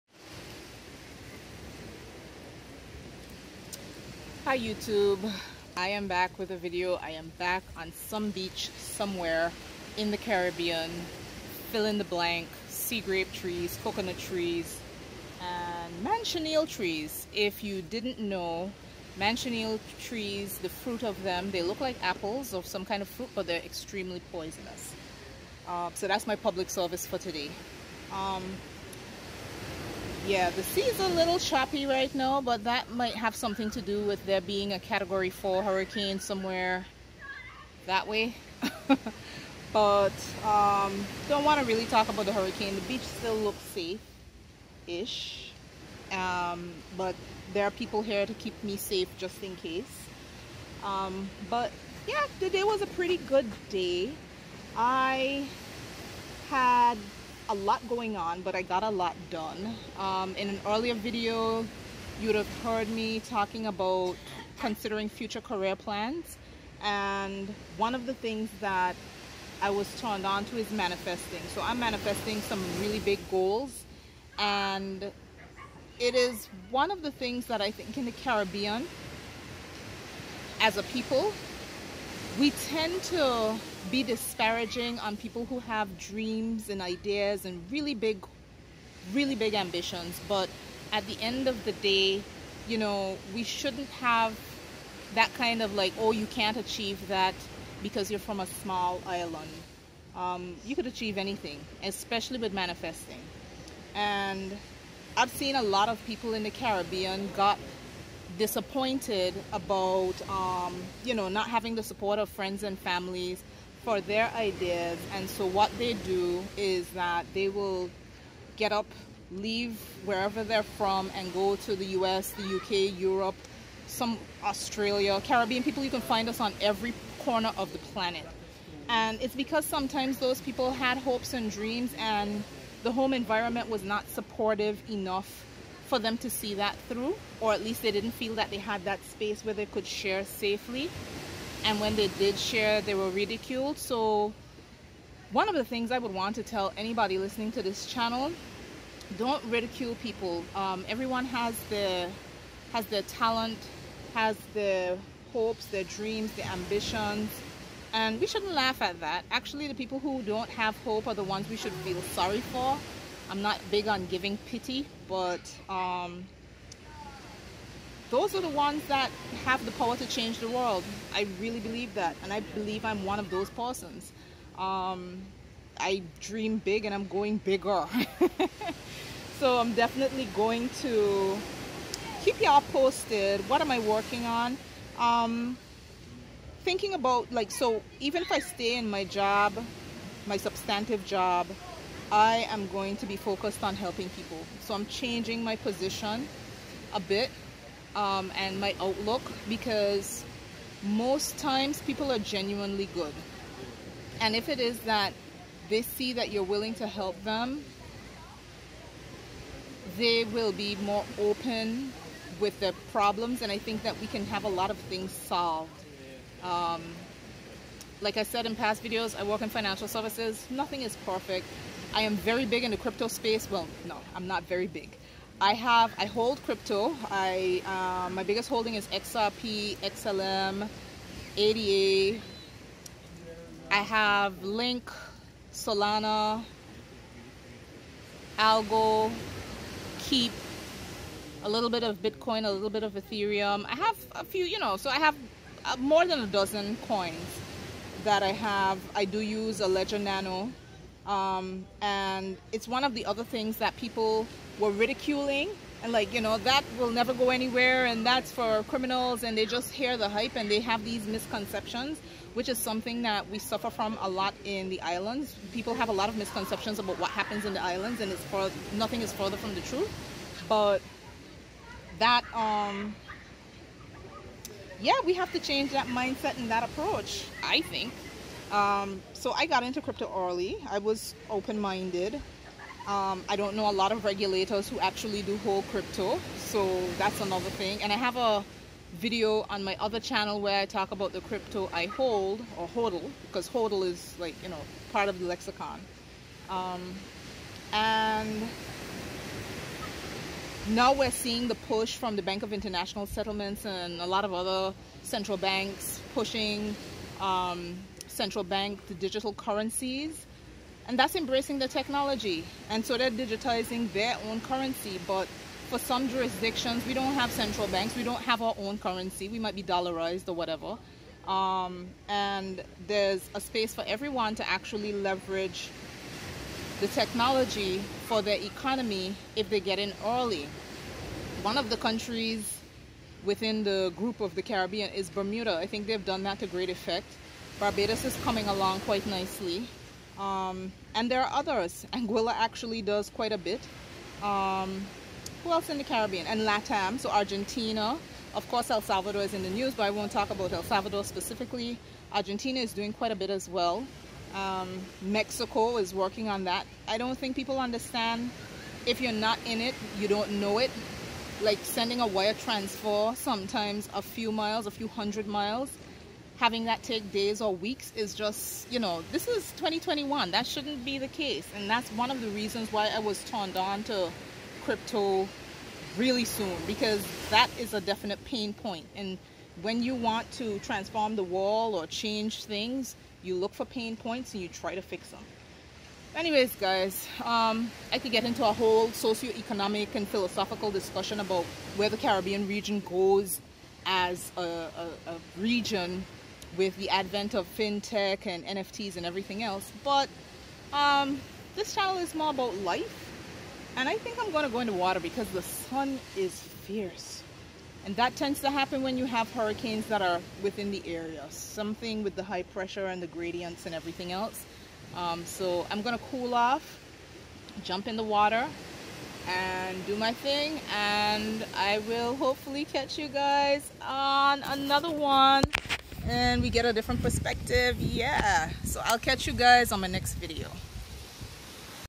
HOPE, THE CARIBBEAN AND CRYPTO!
What may contribute to the Caribbean brain-drain, random crypto chatter and manifesting.